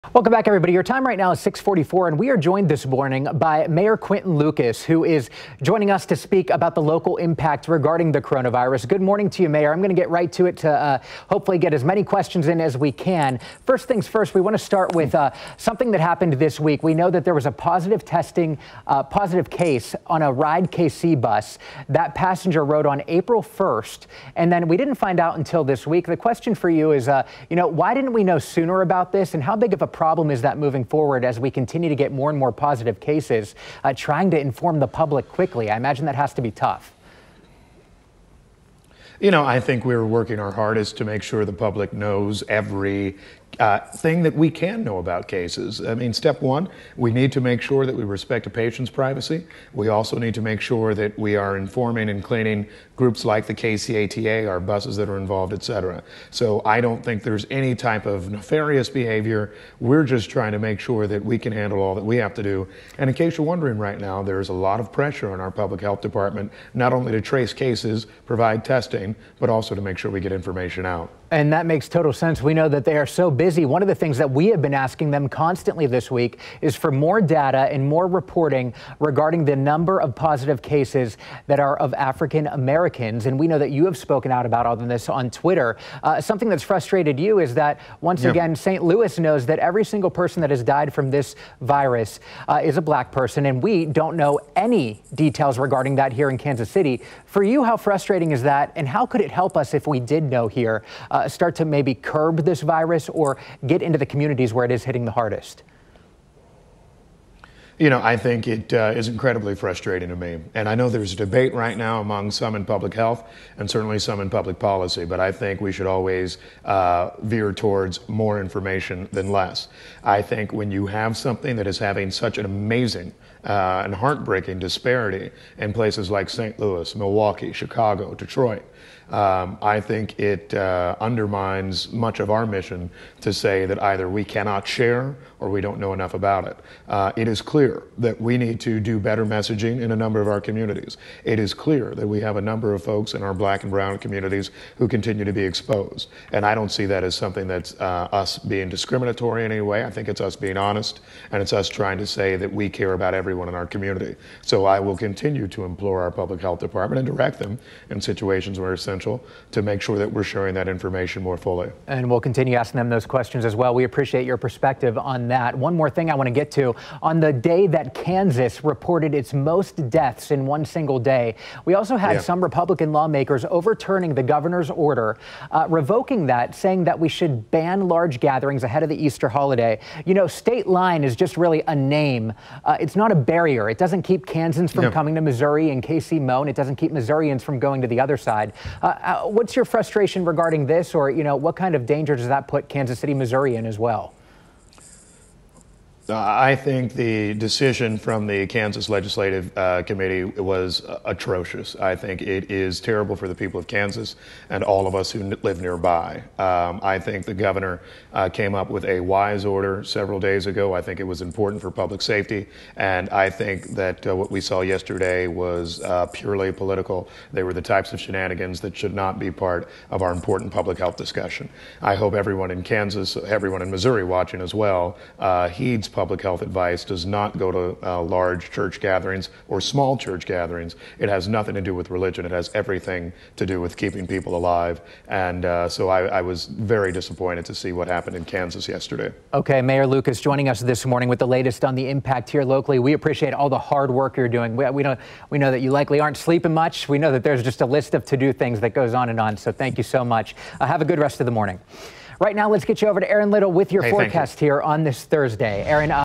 Mayor Lucas wants Missouri to better report amount of black people dying from coronavirus
Mayor Quinton Lucas wants Missouri to better report the amount of black people dying from coronavirus. And, he has some choice words for lawmakers who revoked Gov. Laura Kelly's order banning large gatherings in churches of more than 10 people -- right ahead of Easter.